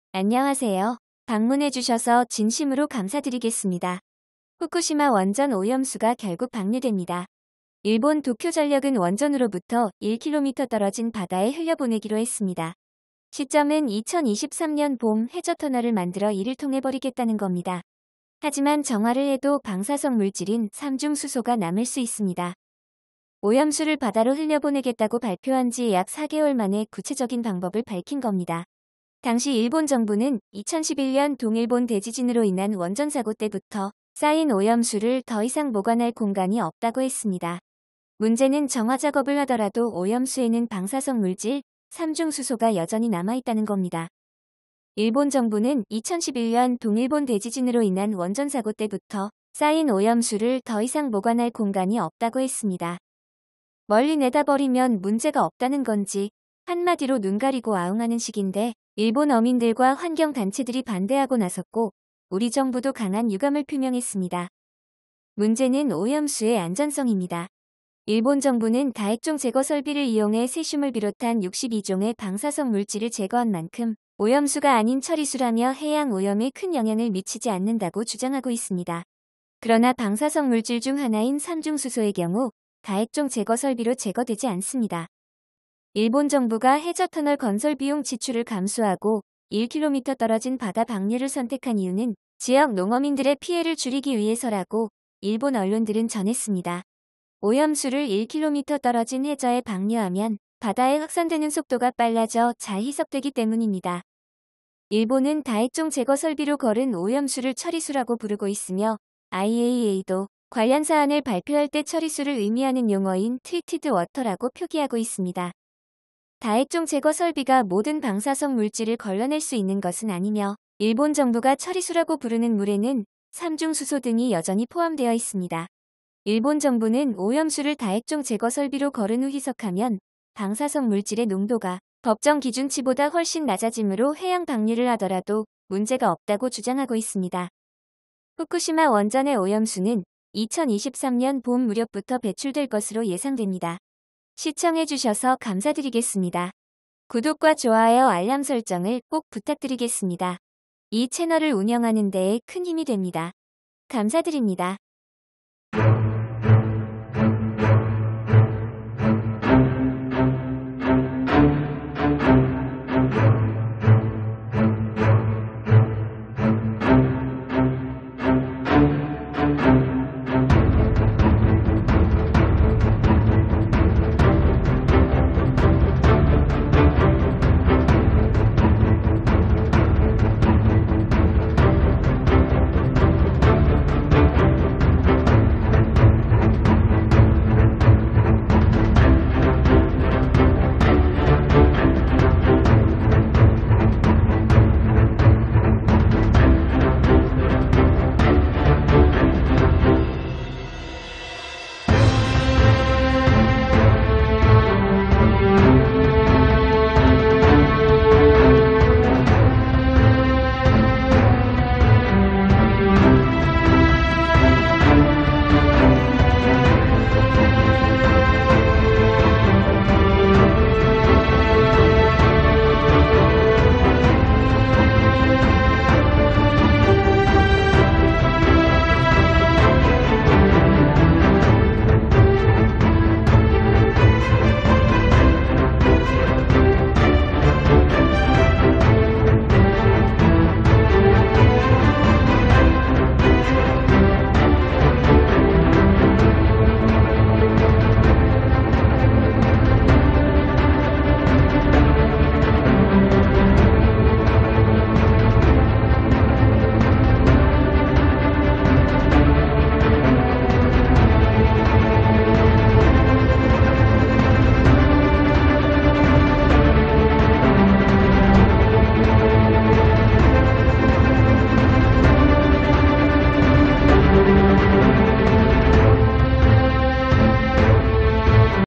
긴급속보 일본 후쿠시마 오염수 결국 전쟁을 선택했다!! 멀리 버리면 괜찮다?…"후쿠시마 오염수 1km 밖에 방류" CLOVA Dubbing으로 제작한 AI 보이스 입니다.
긴급속보 일본 후쿠시마 오염수 결국 전쟁을 선택했다!! 멀리 버리면 괜찮다?…"후쿠시마 오염수 1km 밖에 방류"